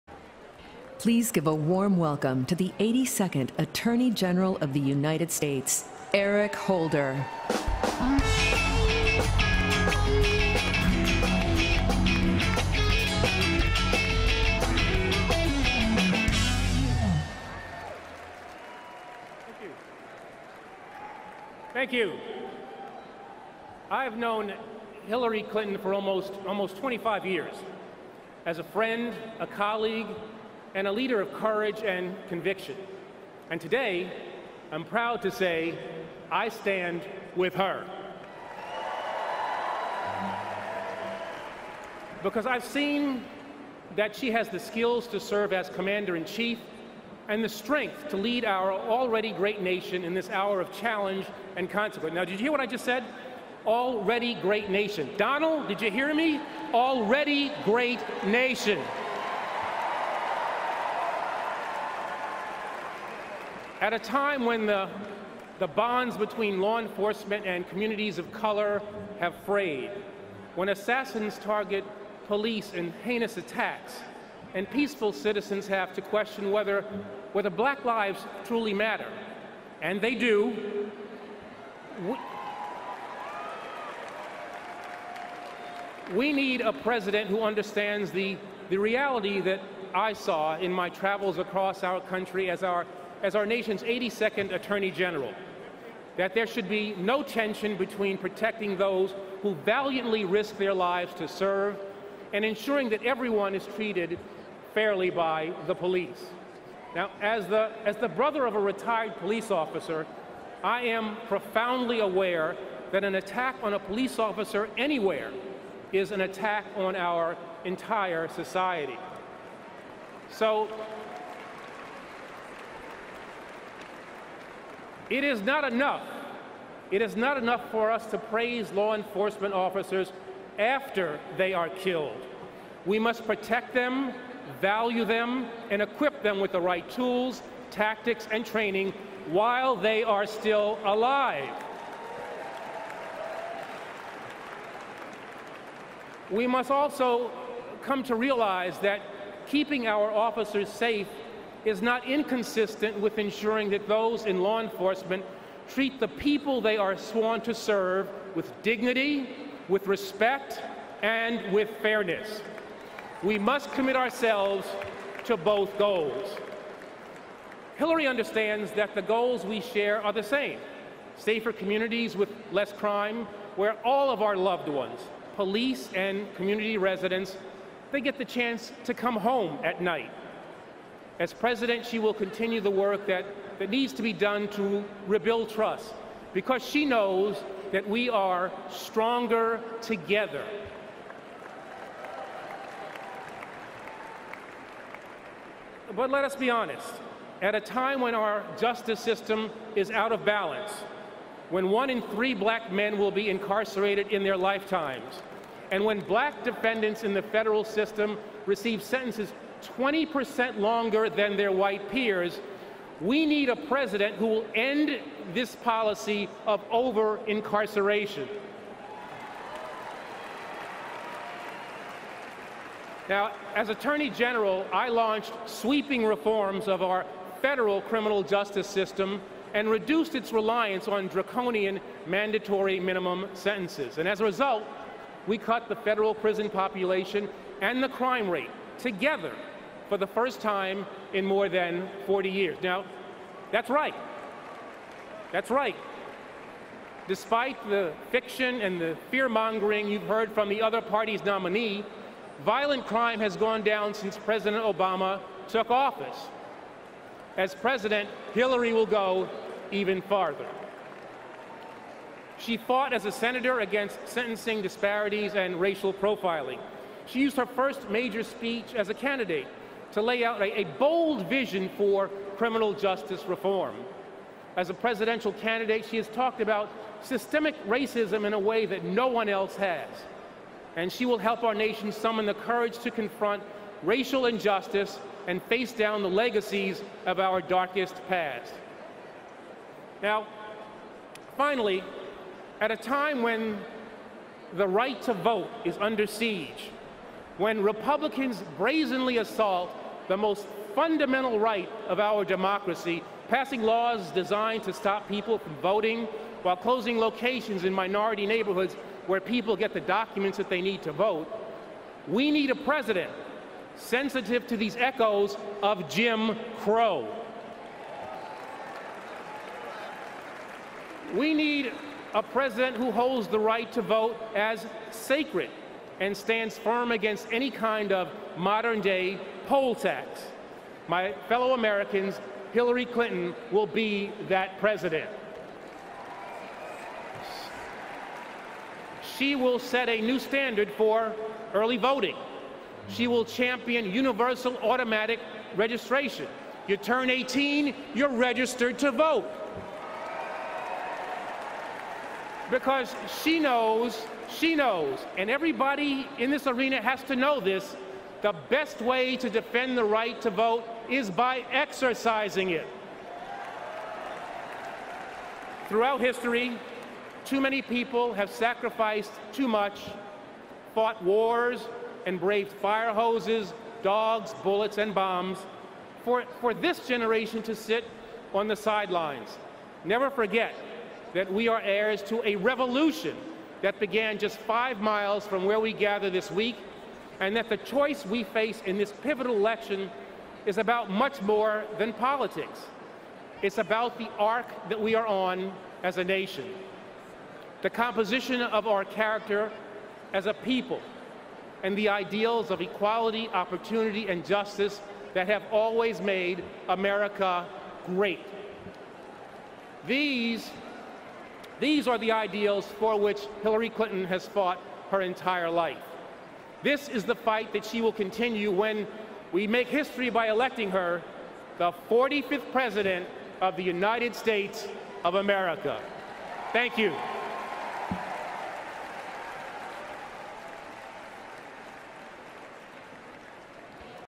Eric Holder at the DNC
Former Attorney General Eric Holder speaks at the 2016 Democratic National Convention